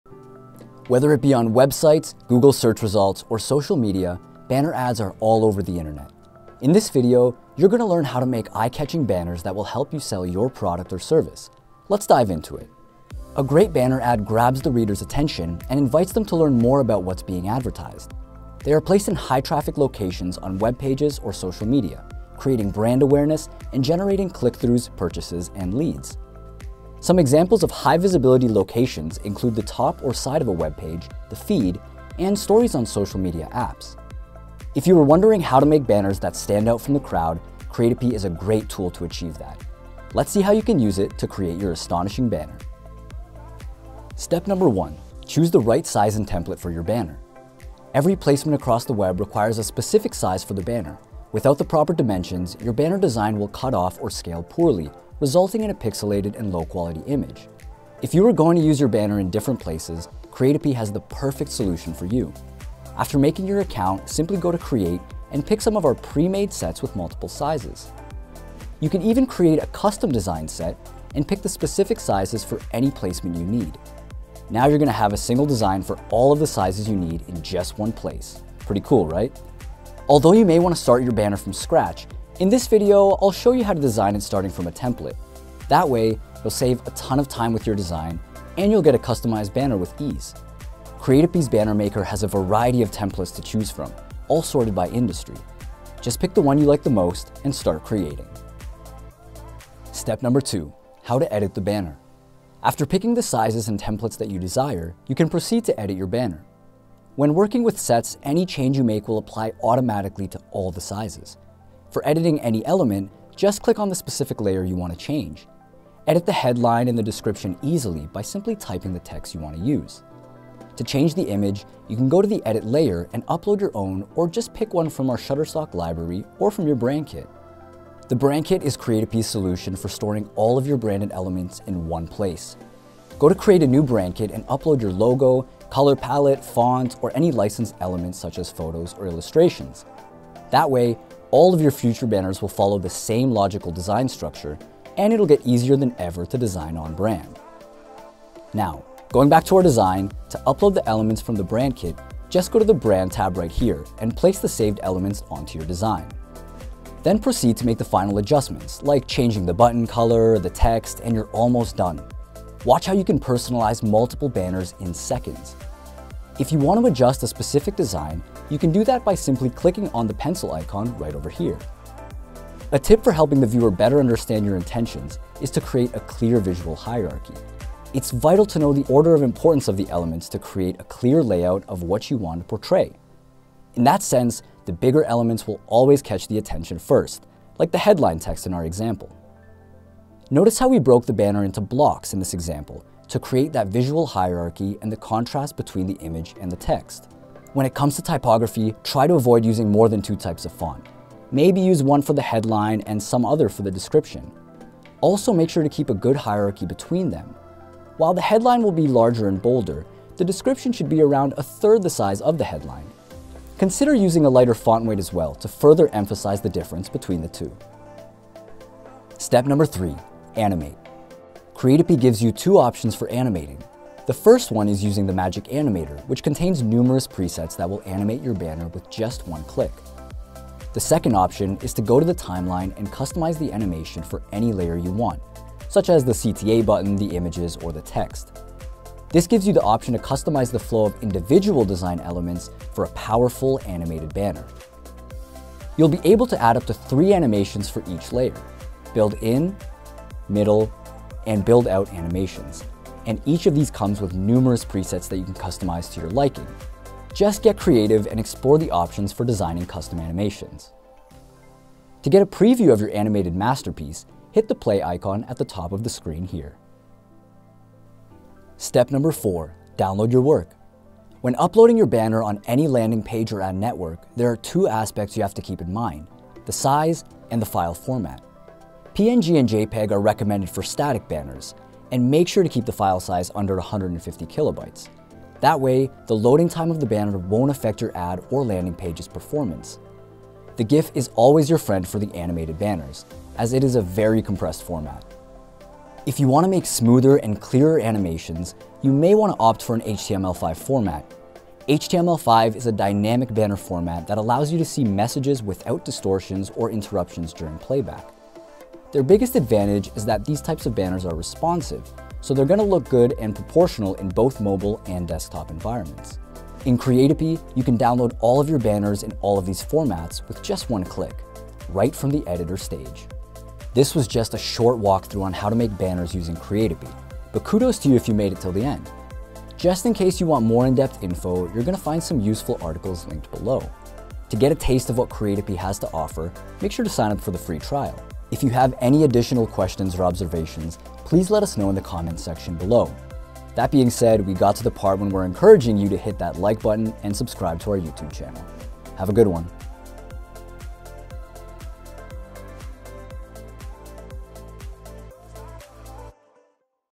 Create Multiple Ad Sets Using the Online Banner Generator
There’s a reason online banner ads are one of the most popular promotion methods to this day; it’s efficient, not to mention cost-effective.

With Creatopy's banner generator, it’s easy to learn how to create banner sets like a professional designer, regardless of your level of experience, and launch campaigns at a faster pace.

If banner advertising isn’t already part of your marketing strategy, you should definitely give it a try.

Follow the easy steps in this video, and you’ll have multiple banner designs for an entire campaign ready to be published and get the word out about your brand in no time.

Video outline
Intro (00:00)
Step no. 1: Choose the right size and template (00:44)
Step no. 2: How to edit the banner (1:40)
Step no. 3: Animate the design (3:39)
Step no. 4: Download your work (4:29)
Outro (5:30)